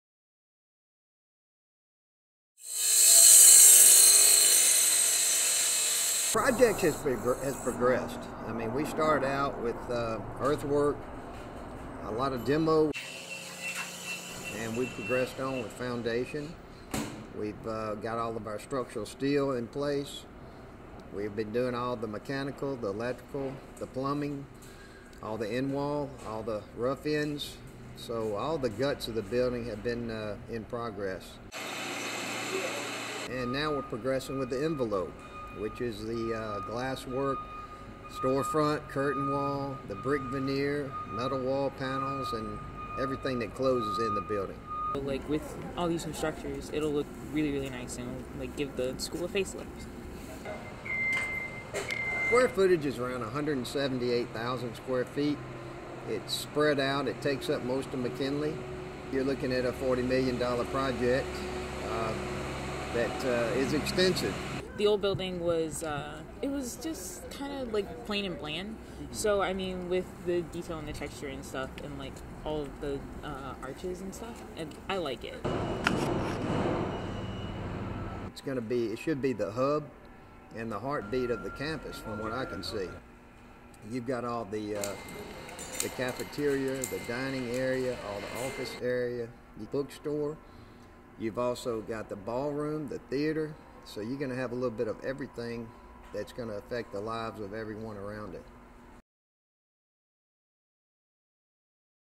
Student Union Construction Update
Construction crews are making significant progress toward completing the Student Union. Project Superintendent Jack Craft says, "This will be the hub and the heartbeat of the campus. You have the cafeteria, dining area, ballroom, theater and a little bit of everything that is going to affect the lives of everyone around it."